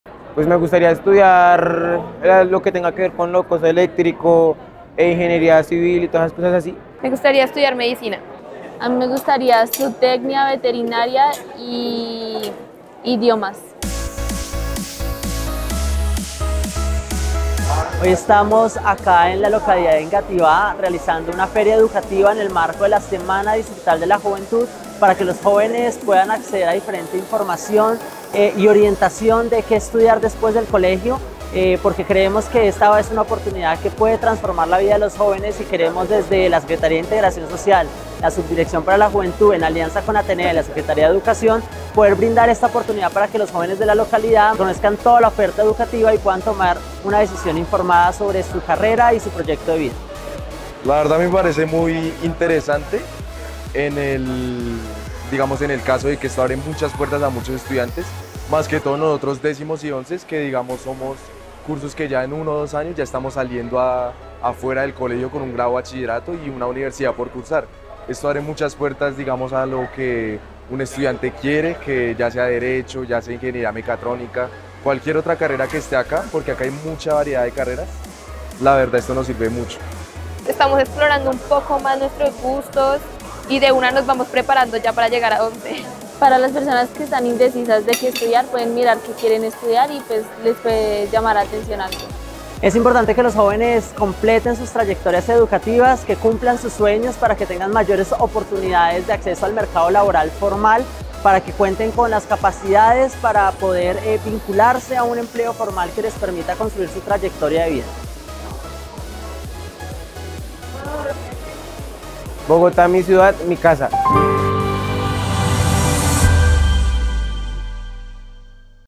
Las oportunidades para la juventud continúan, esta vez estuvimos en la localidad de Engativá
Más de 10 universidades estuvieron presente en la gran feria universitaria llevada a cabo en el 'Colegio Nueva Constitución', allí los jóvenes de la localidad pudieron explorar nuevas oportunidades de estudio para después de su grado.